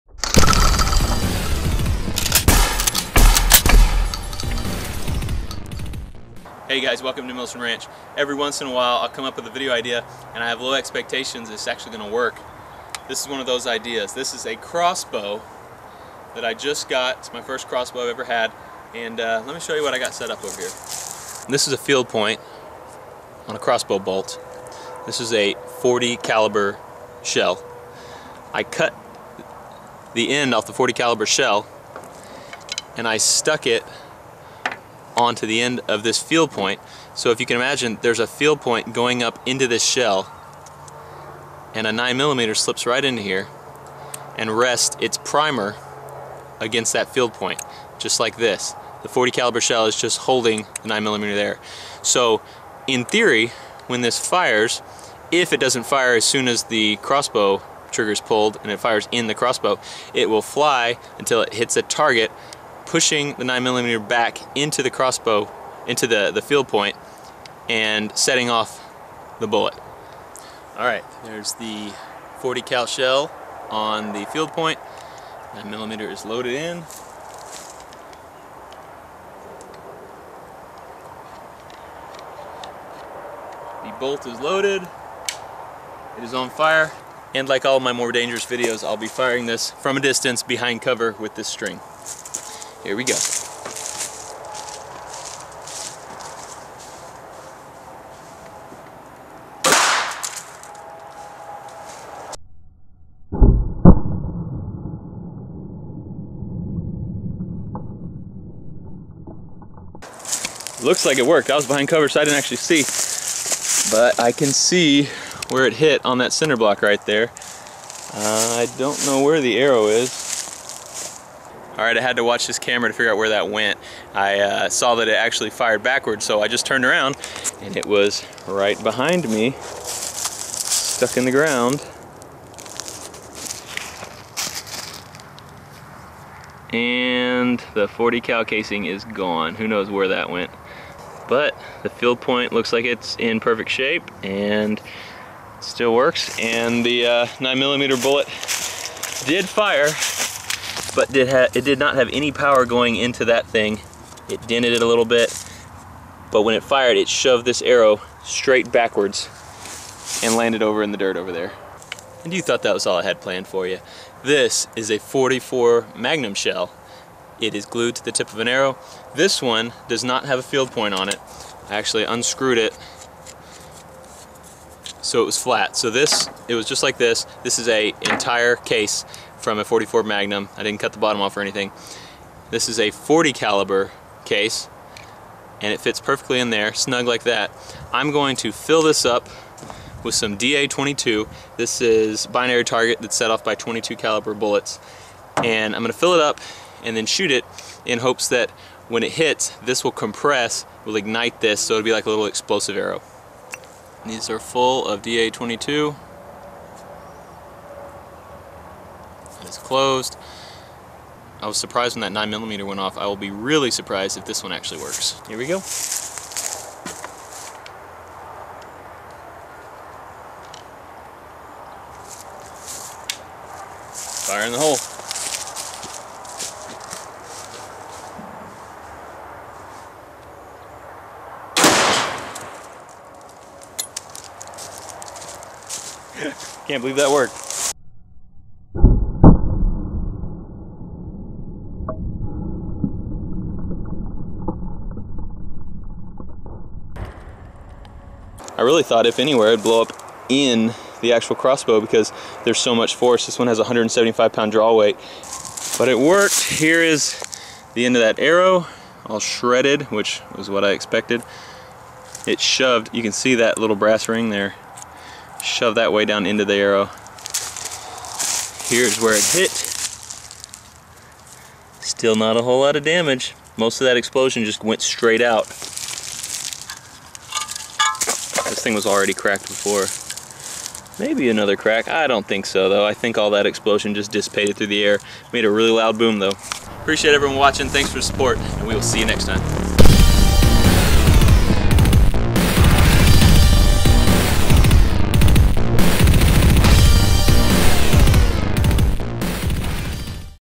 Explosive Tipped Crossbow Bolt
Testing some homemade explosive tipped arrows.